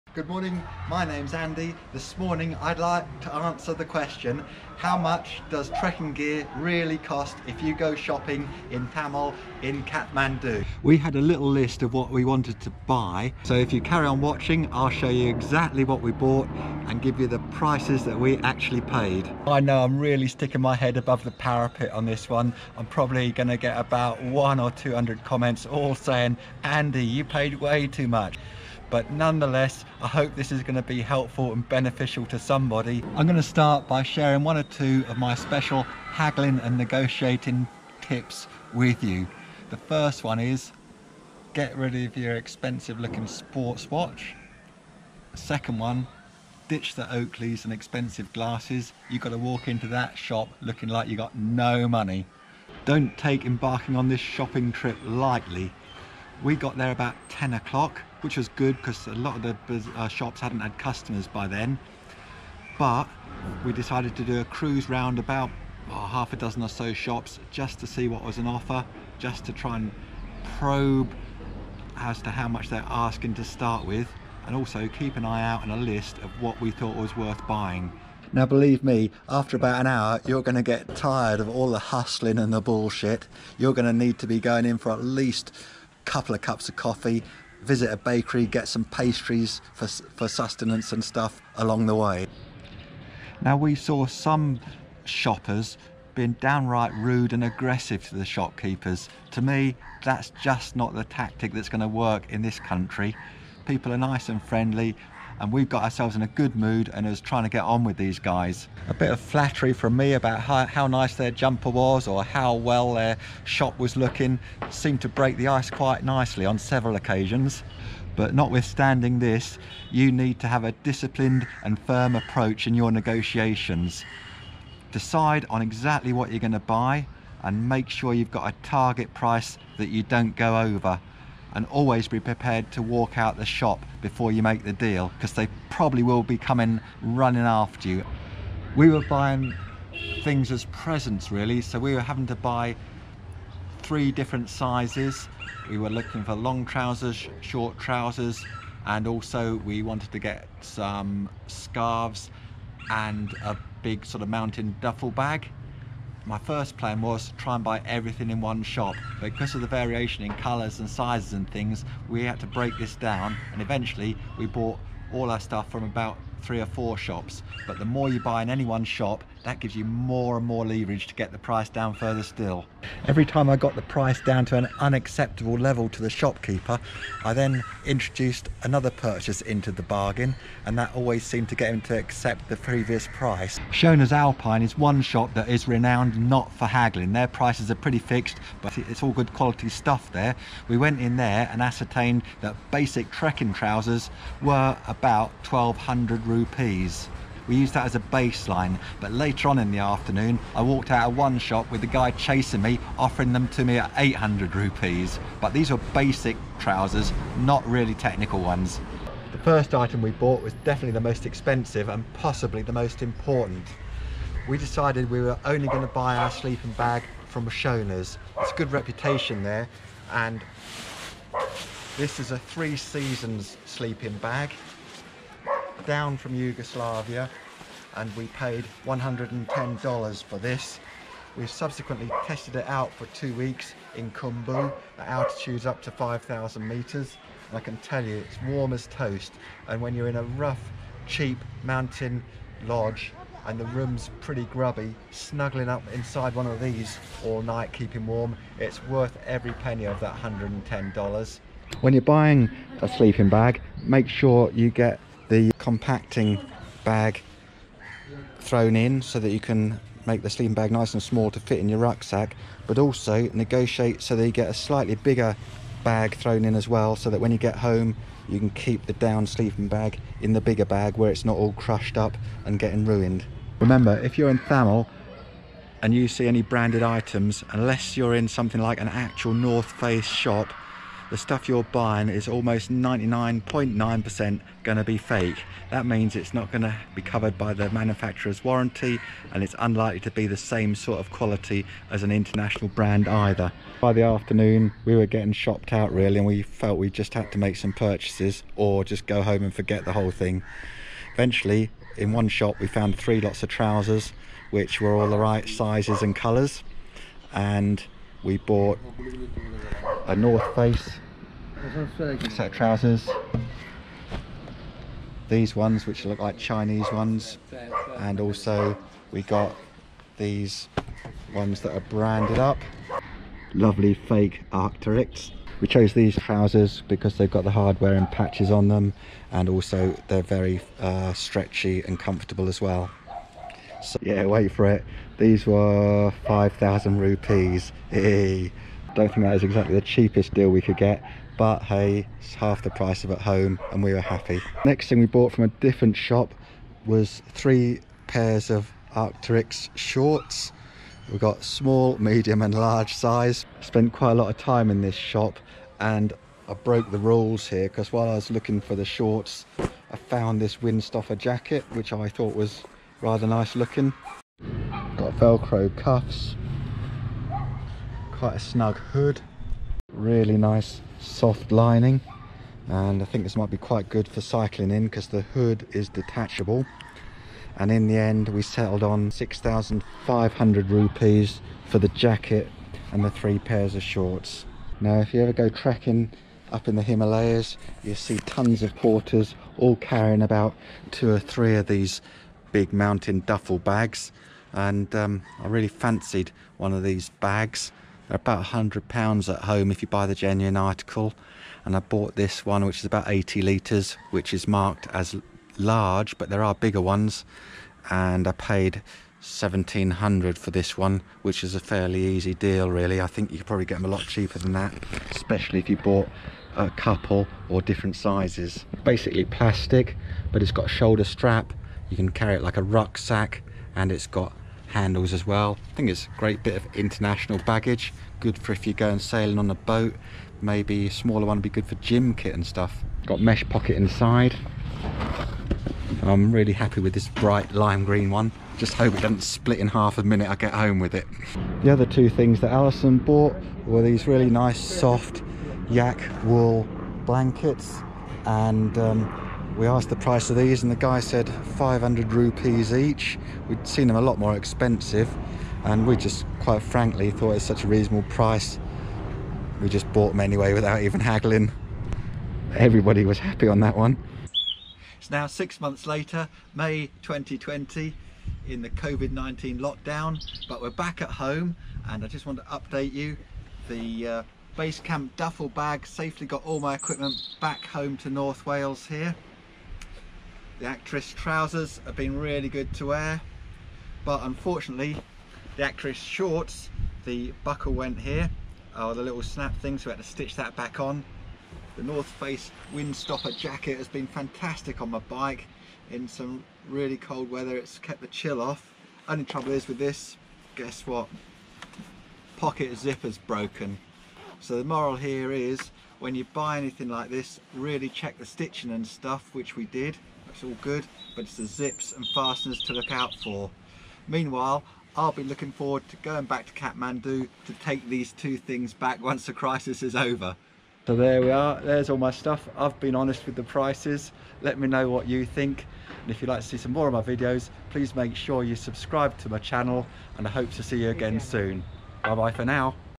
Shopping for Trekking Gear in Thamel, Kathmandu. What Prices Should I Really Pay?
Cheap, fake trekking gear is hard to resist buying in Thamel, Kathmandu. Shopping here is an experience on its own with no marked prices you can expect lots of bartering and good natured haggling. Establishing what is a good product and what is a keen price is not always straightforward. Here's how to avoid a few pitfalls and increase your odds of getting some good bargains. Jackets, trousers, poles, socks, boots, sleeping bags you can get anything you need here but if you're life's going to rely on it up in the mountains you're definitely best buying it before you leave home!